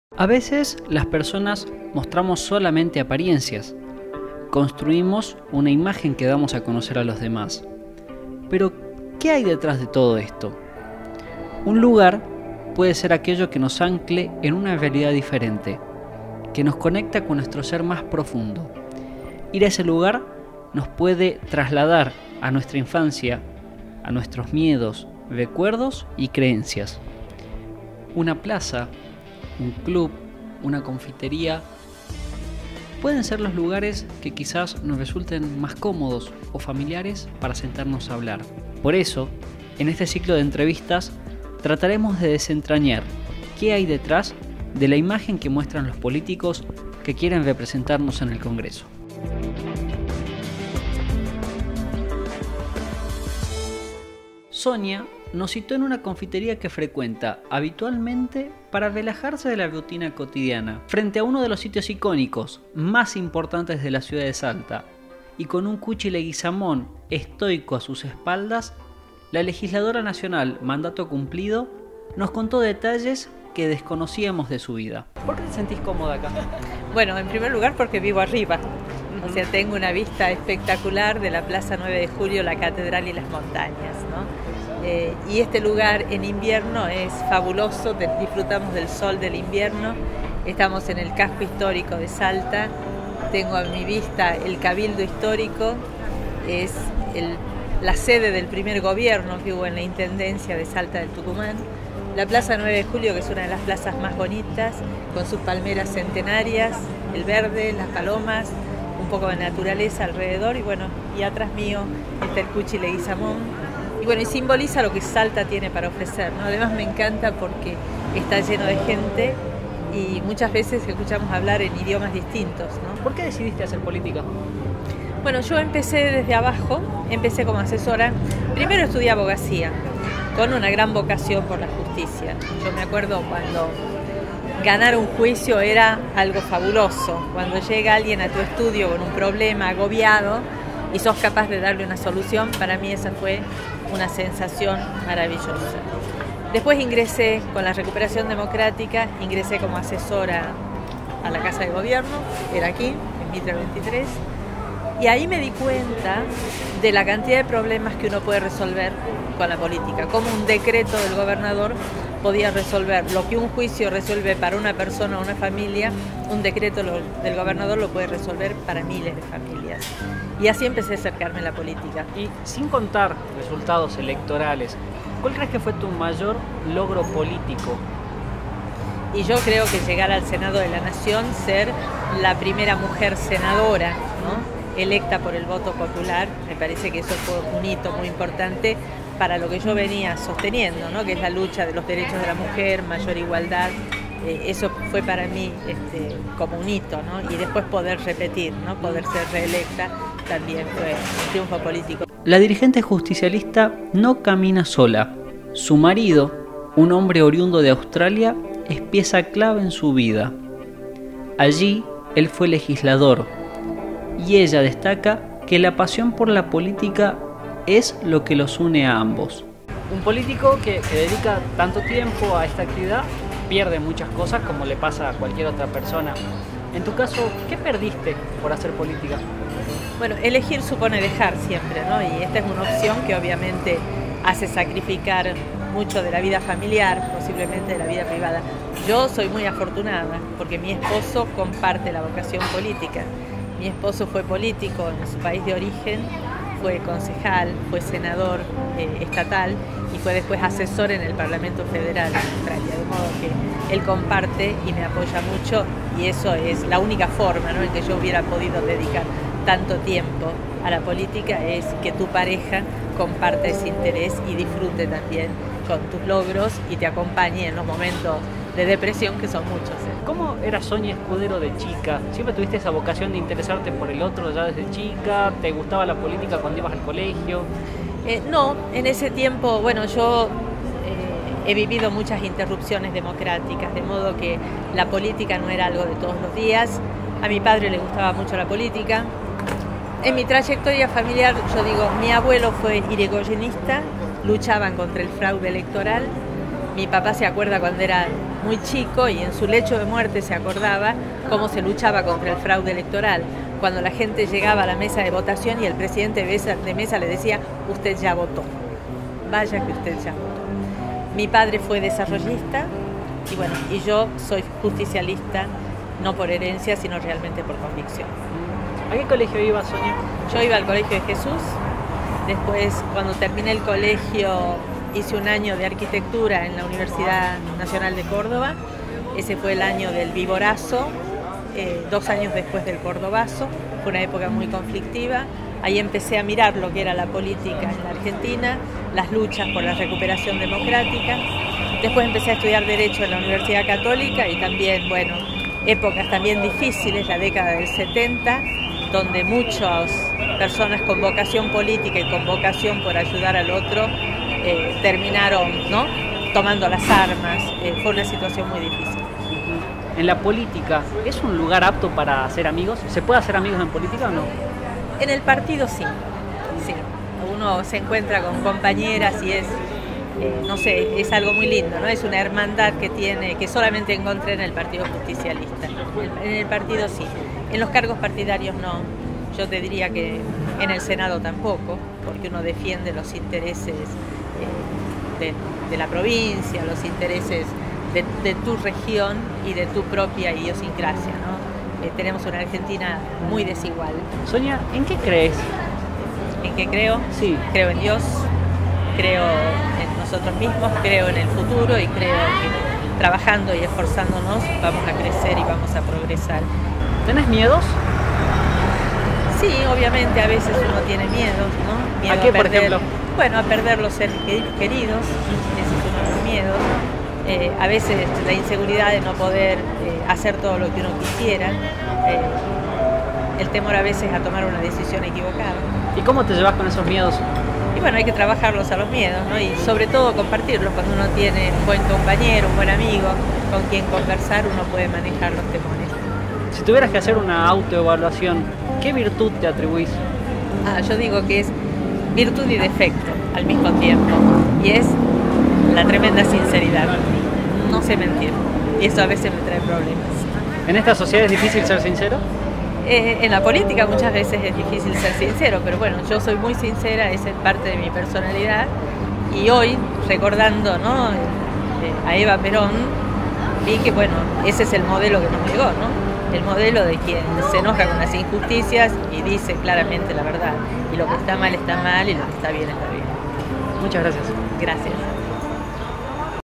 MI LUGAR: ¿Quién es Sonia Escudero?
En este ciclo de entrevistas a los pre candidatos legislativos te contamos detalles de la vida de la primera mujer en ser senadora nacional por Salta.